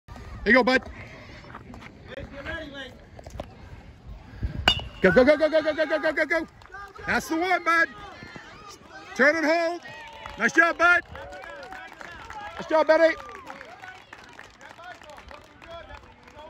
First RBI of the victory 6/17
Ben singled through the left side of the infield to plate Lincoln Latuscha for the game's first run.  Auburn 10U All Stars scored early  and often against Mid-coast for a 13-3 "mercy rule" after 3 1/2 innings.